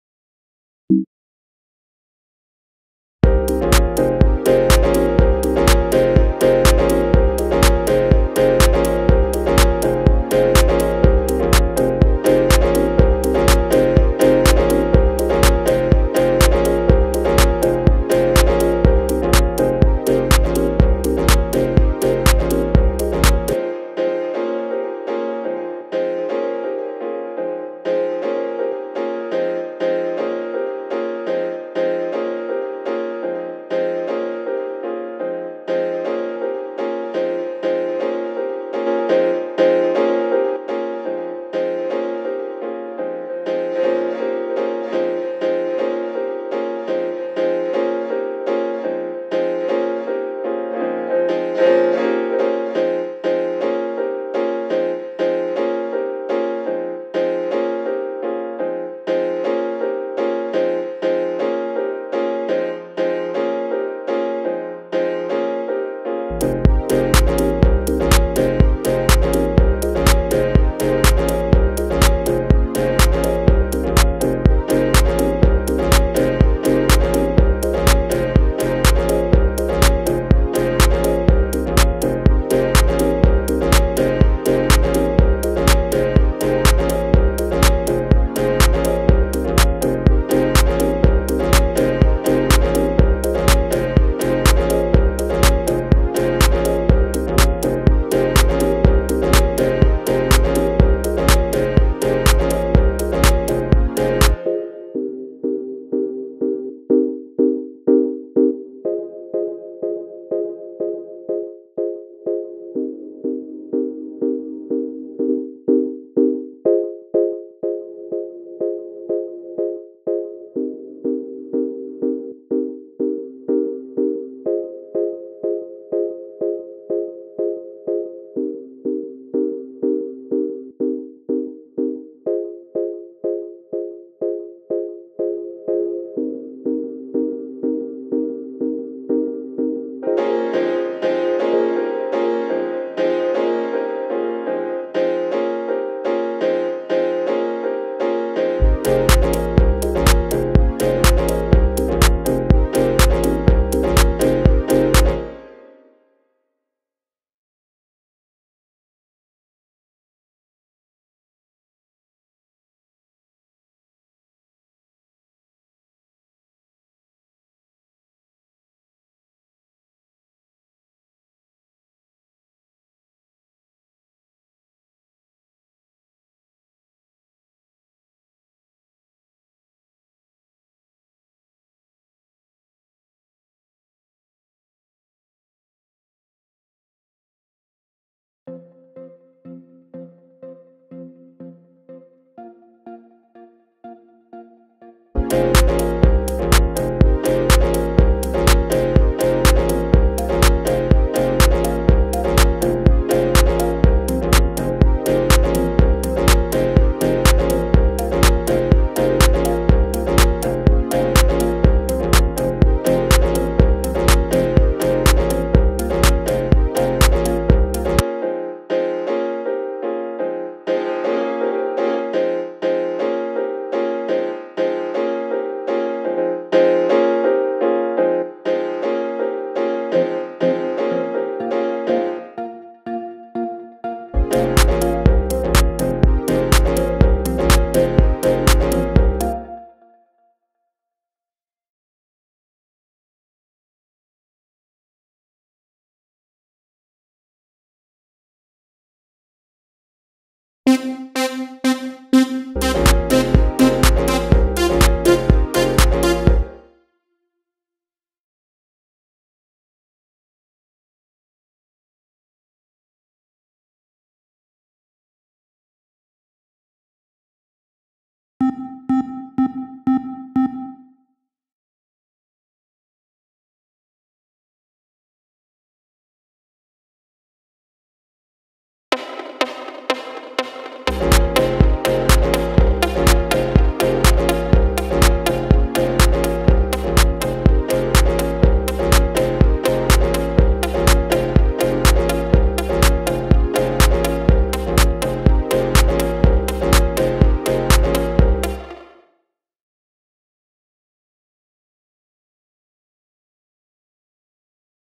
Trying out GA Reverb and SD Channel presets
Grant Nelson gave away some classic Lexicon Reverb Impulse responses here is a video of trying a few of them out, also some Channel presets I got from Loopmasters by Scott Diaz
You can find both here